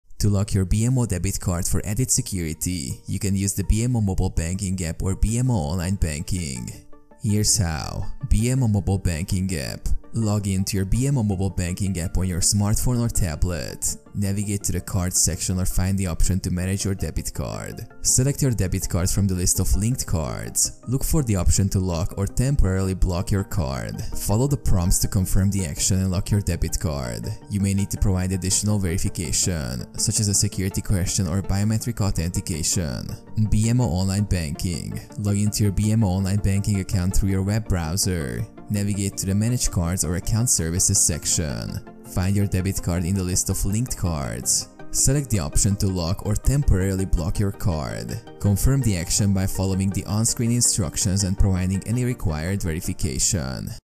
How To Lock BMO Debit Card In 2024?
In this video I will explain How You Can Lock BMO Debit Card In 2024.